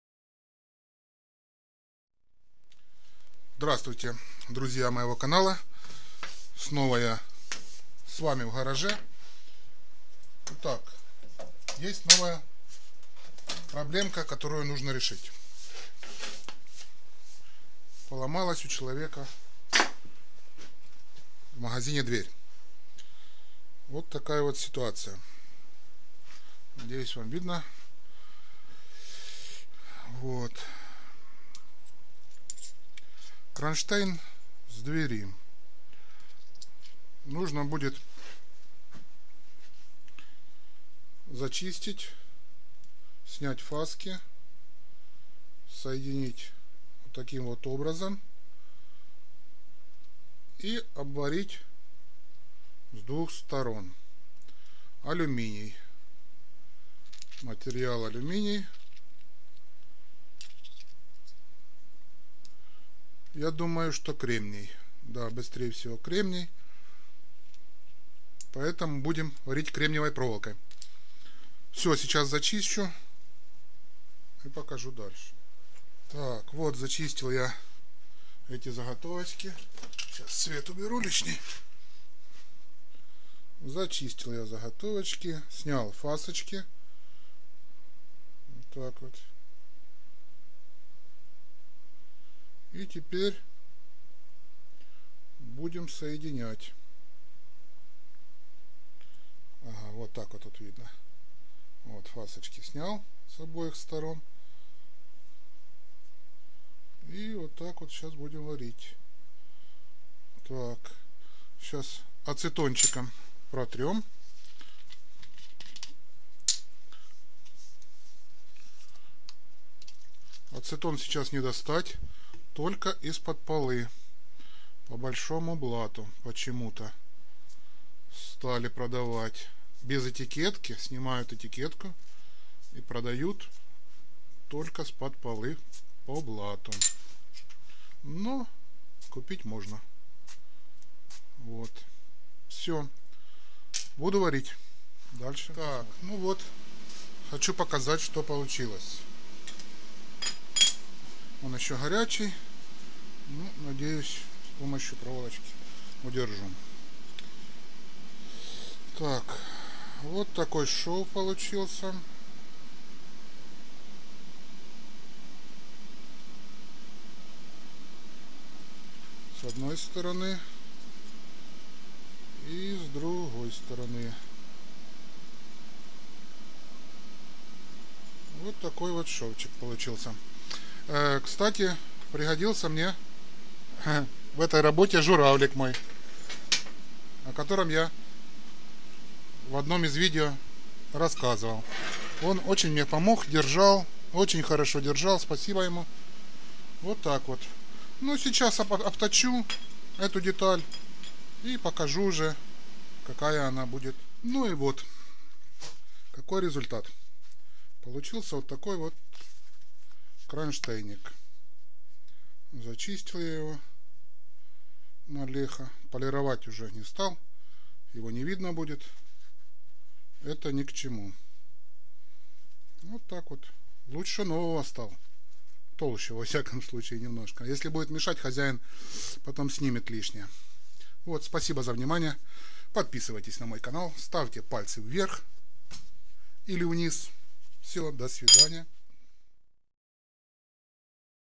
АРГОНОВАЯ СВАРКА ДЮРАЛЮМИНИЯ.
Ремонт дюралюминевого кронштейна двери.